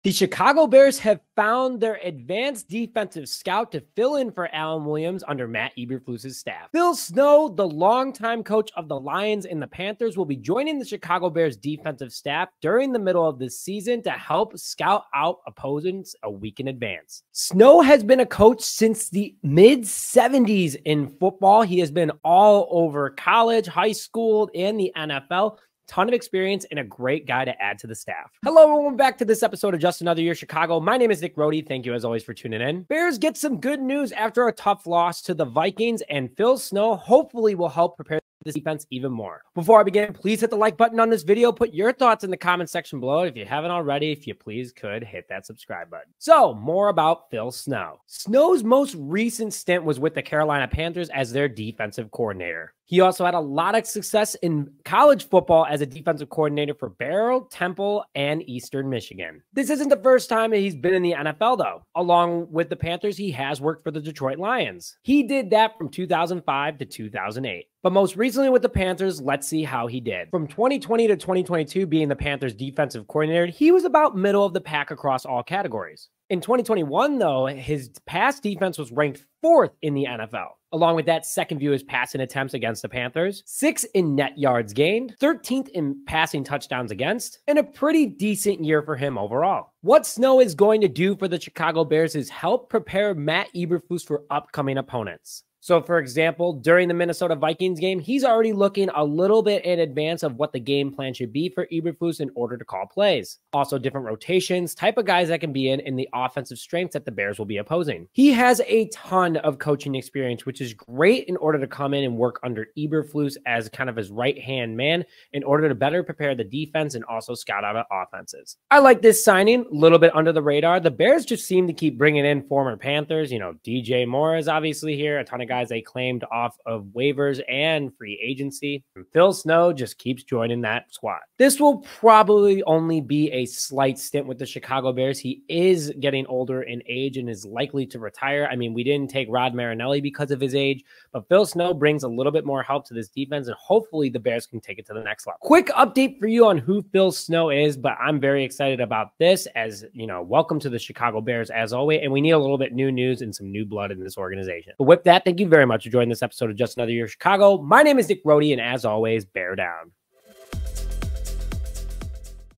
Chicago Bears Sign Phil Snow As a Senior Defensive Analyst. Chicago Bears Updates
The Chicago Bears have found their a Senior Defensive Analyst in Phil Snow.

Snow joins Matt Eberflus' staff nearly halfway through the year after former defensive coordinator Alan Williams resigned due to an HR issue with The Chicago Bears.

Snow has a long history of coaching and is looking forward to beginning his new chapter with The Chicago Bears.

Welcome to Chicago, Phil!